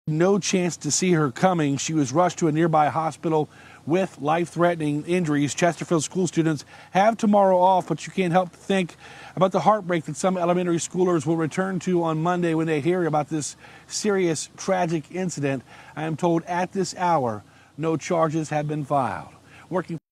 Child seriously injured after being hit by truck in Chesterfield
Crime Insider sources said the child who was hit was a 10-year-old girl.